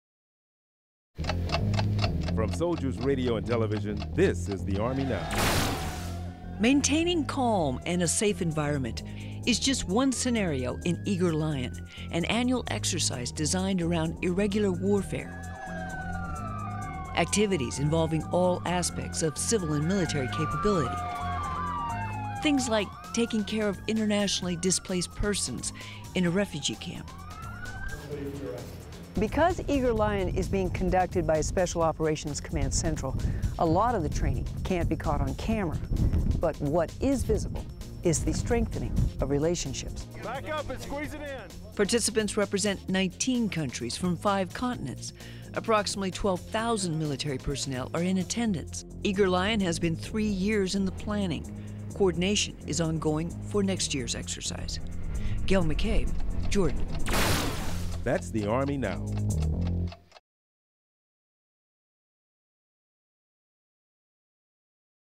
Special Operation Trains Northern African Soldiers
Special Operations train Northern African Soldiers from five countries during Eager Lion 2012 in Jordan